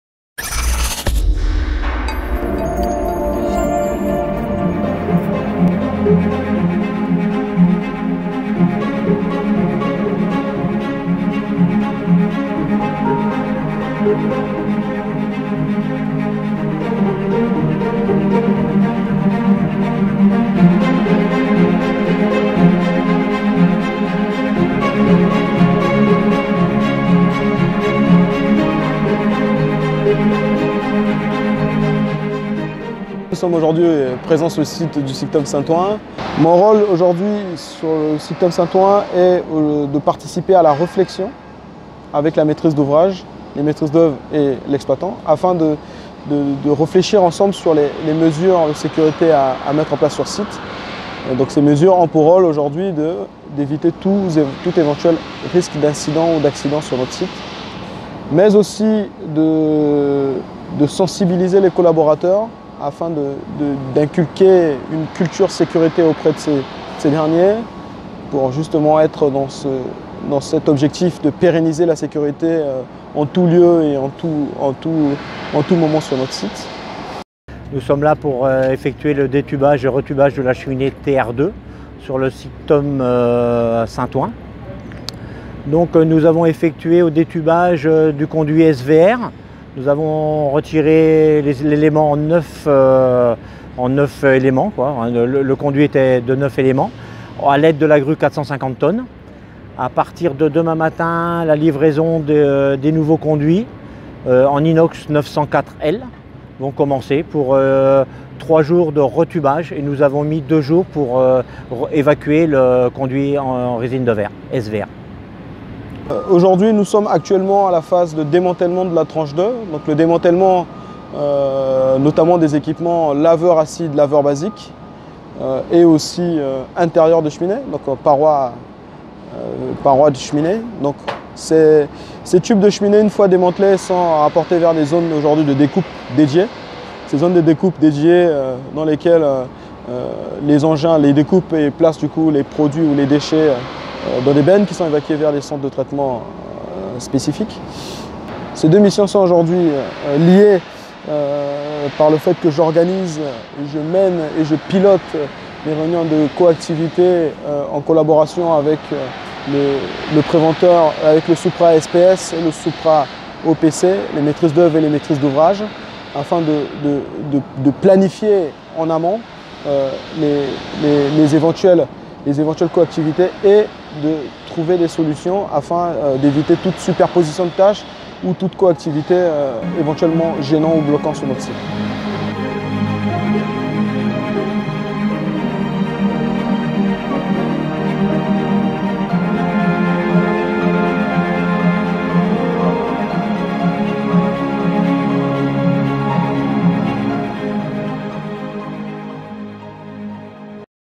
BECS - Mission de coordination SPS Conception et Réalisation au SYCTOM Traitement des fumées St Ouen
BECS Agence Ile de France assure la mission SPS des travaux de requalification de traitement des fumées du centre de traitement des déchets ménagers SYCTOM à Saint-Ouen, le site reste en activité pendant toute la durée du chantier.